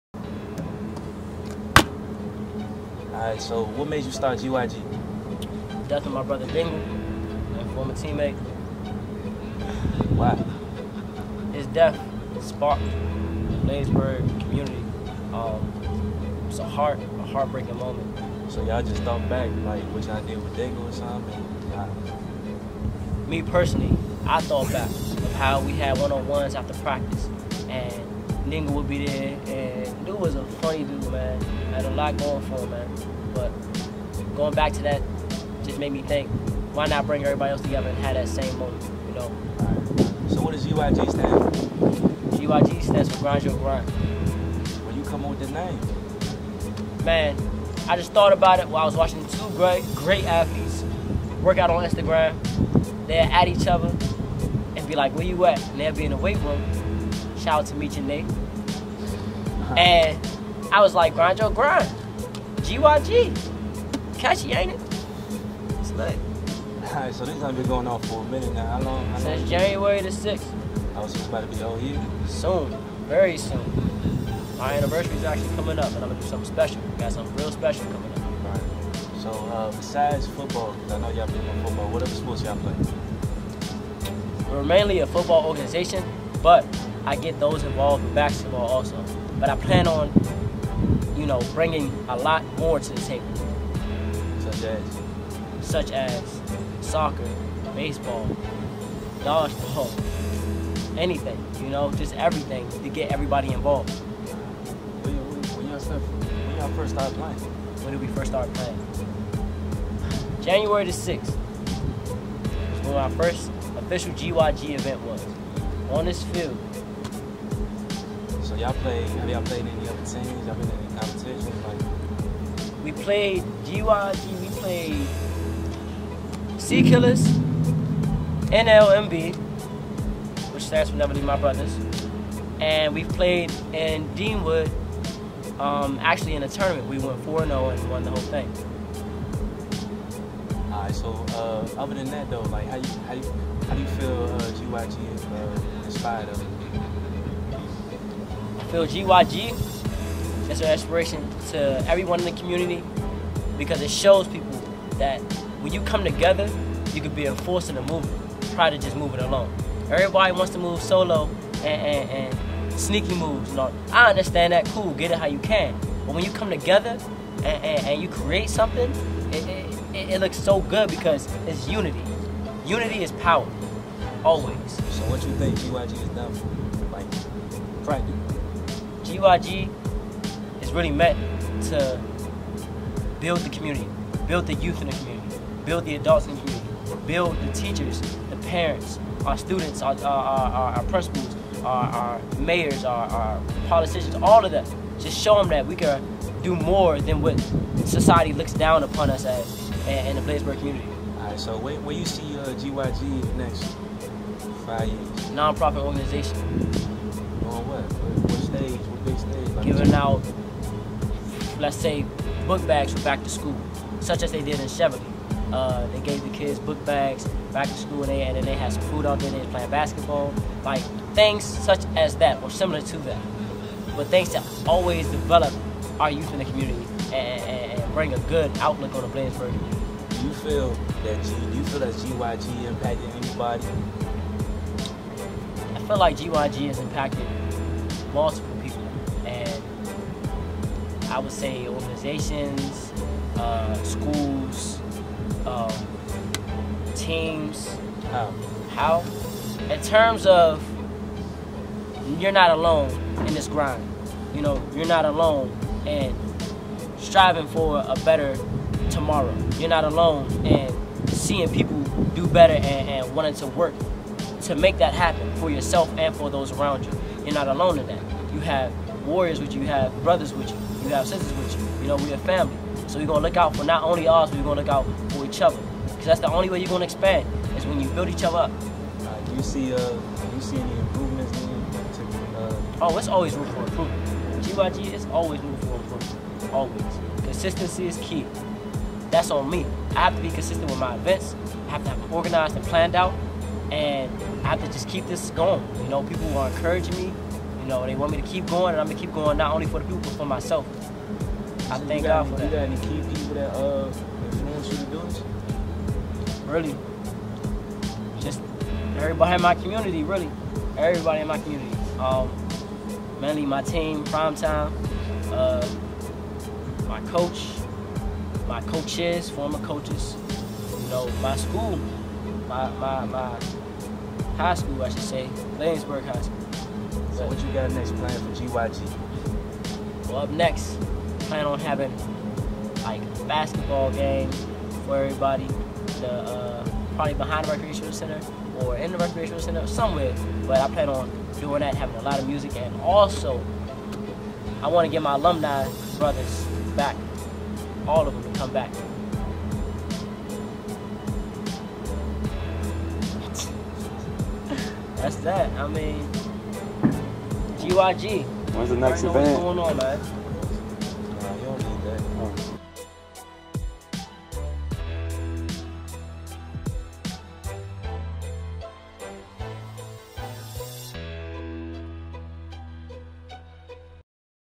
GYG Short documentary
This is a Short Documentary of GYG and it’s success and plans so far with in 2019 . I Howard Coles III Am speaking on the behalf of my team and organization . We have a lot more videos and moments that haven’t been displayed . I would like to see everyone become more active in the GYG Community in order to make a positive change . GYG Grind Your Grind does not only consist of just sports you Grind Your Grind in the everyday life as a human being such as grinding at work , for your family , school , careers , etc . It is Your Grind and no one else can stop you but you .

GOD ❤️
GYG 🖤
Primetime 🏈
Coach O 🏀 (BHS Basketball Head Coach)
MoveForMental 🧠
Bladensburg High School 🐎
Bladensburg Alumni🐎
Everyone who represented there school
( PG County , DC , Montgomery County , and Howard County )

‼️ Edits By @Scriptedvisuals ‼️